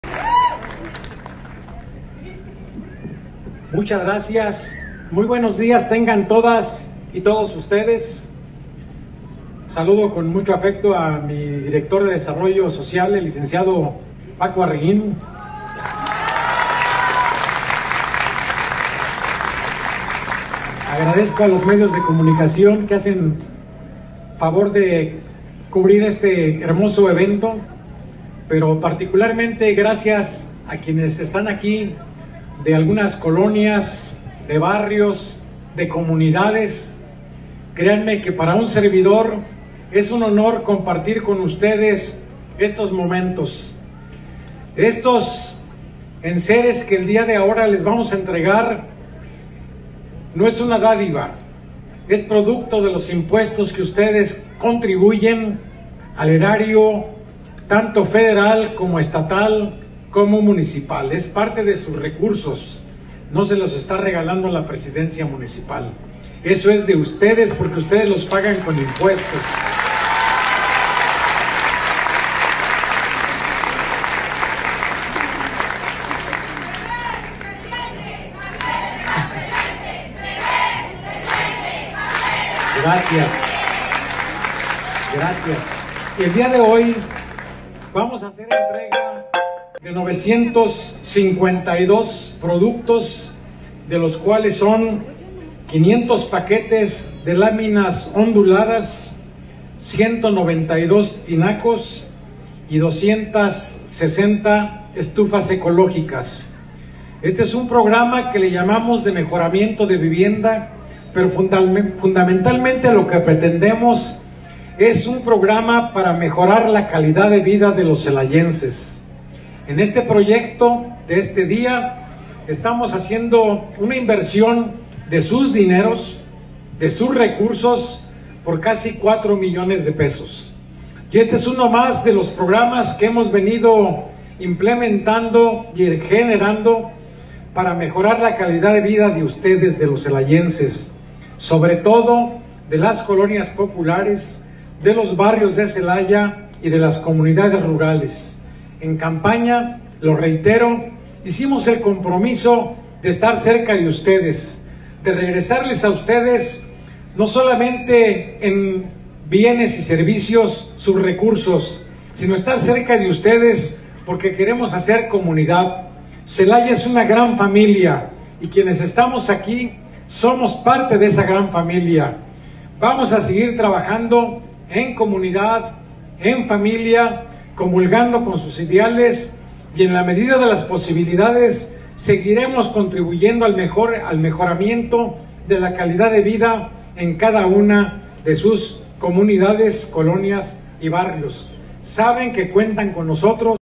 Programa Mejoramiento de Vivienda: entrega de tinacos, láminas y ecoestufas
Celaya 👥️Programa Mejoramiento de Vivienda: entrega de tinacos, láminas y ecoestufas, mensaje del alcalde, Javier Mendoza